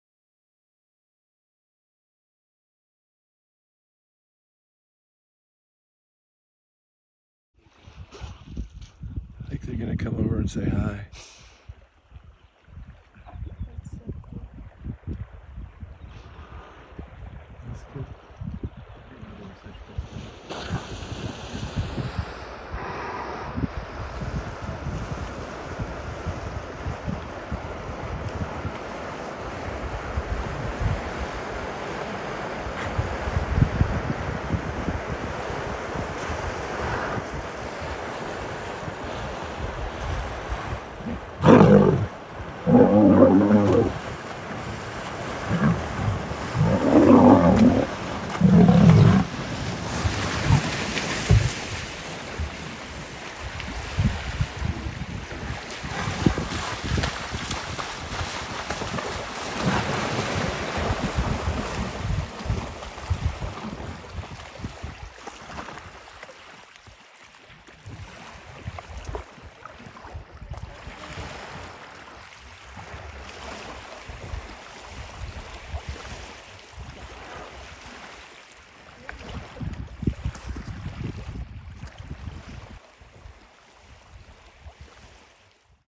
Hippo chases lion at Selinda Explorers Camp | Great Plains
The best safaris are made of unforgettable moments… and this was undoubtedly one of them. Guests at Selinda Explorers Camp in Botswana recently watched as four lions attempted to cross the Selinda Spillway but were intercepted by a hippo. The territorial behemoth moved in at pace to see off the lions, and an incredible interaction between big cats and the mega mammal played out right under the noses of our lucky guests. Thankfully, all involved made it out unscathed.

We feel privileged to have witnessed just another remarkable GreatPlains safarimoment.⁣
Thanks to guest Jon Leman for this fantastic footage.⁣⁣